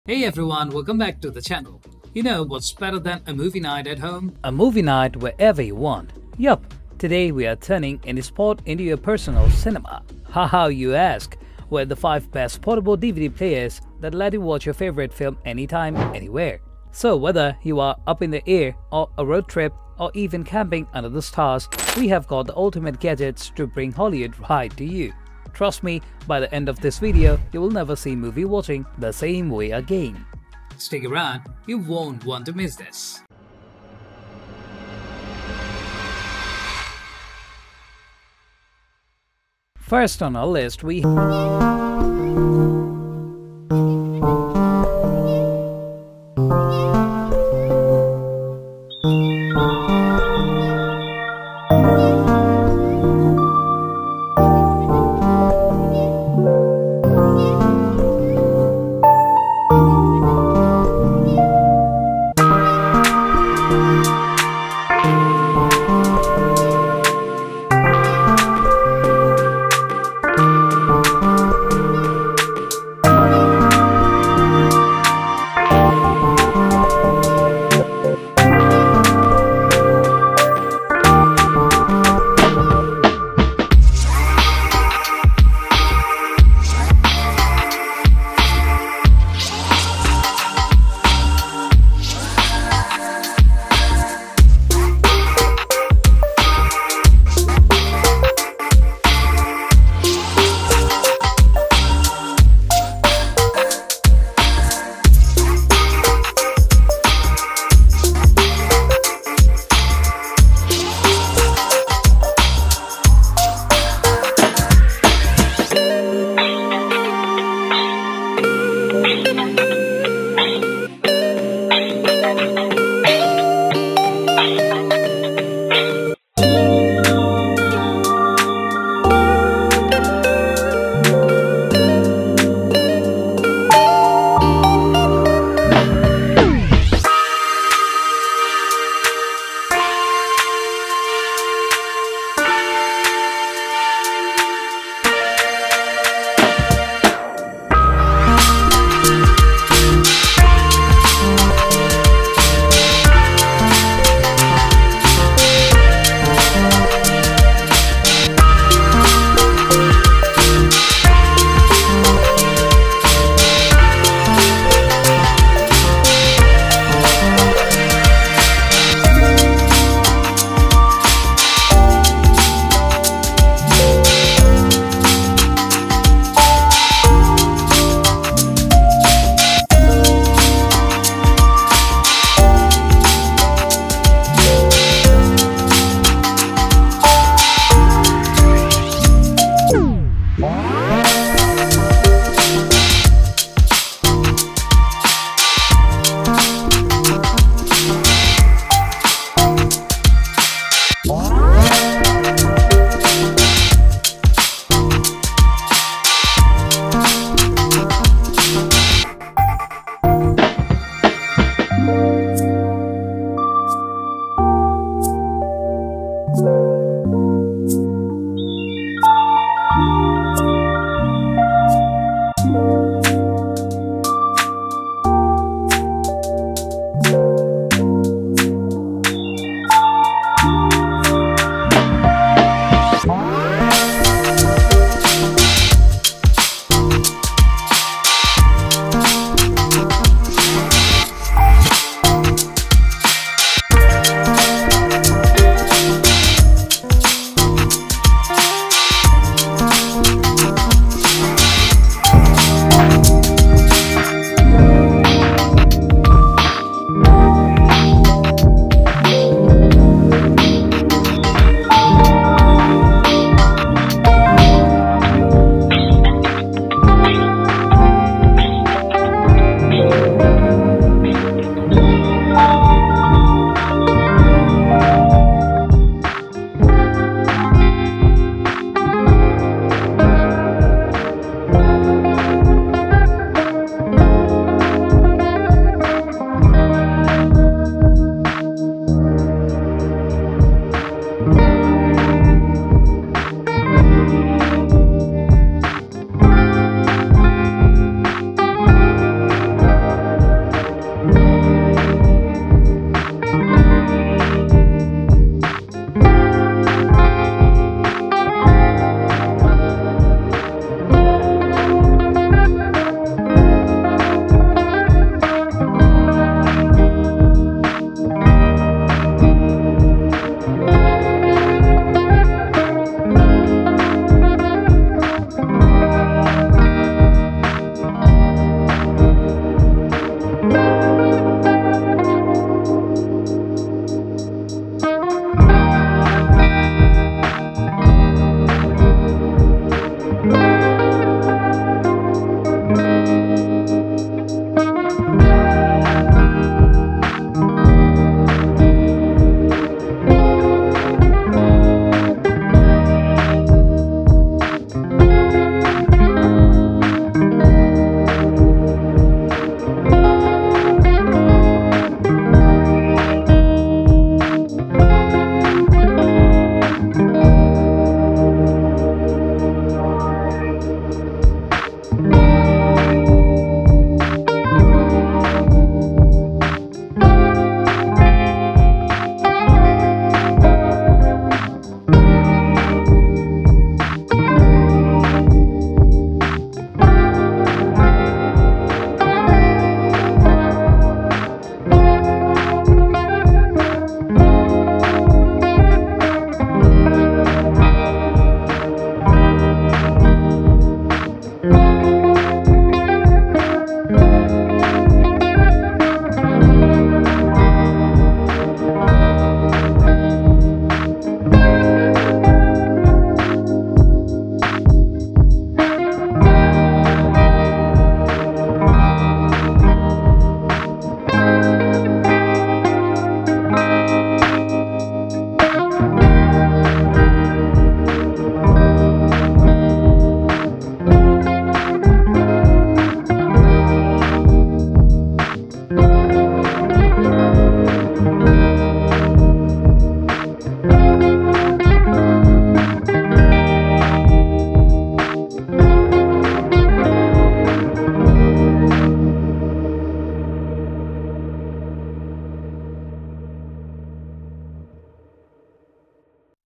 5 Best Portable DVD Players in 2024: Watch Movies Anywhere, Anytime!
Links to the Best Portable DVD Players we listed in today's Portable DVD Player review video:

1 DBPOWER MK101 Portable DVD Player

2 DBPOWER 11.5" Portable DVD Player

3 HD JUNTUNKOR Portable DVD Player

4 ieGeek 11.5" Portable DVD Player with SD Card Slot

5 DR. J 17.9" Portable DVD Player with Large HD Screen

What are the Best Portable DVD Players in 2024?

In today's video we reviewed the Top 5 Best Portable DVD Players on the market in 2024. We made this list based on our personal opinion and we ranked them in no particular order, after doing our research based on their prices, quality, durability, brand reputation and many more.

After watching this video you will know which are the best budget, top selling and top rated Portable DVD Player on the market today.

If you choose from this list you can be sure that you buy one of the best Portable DVD Players available today.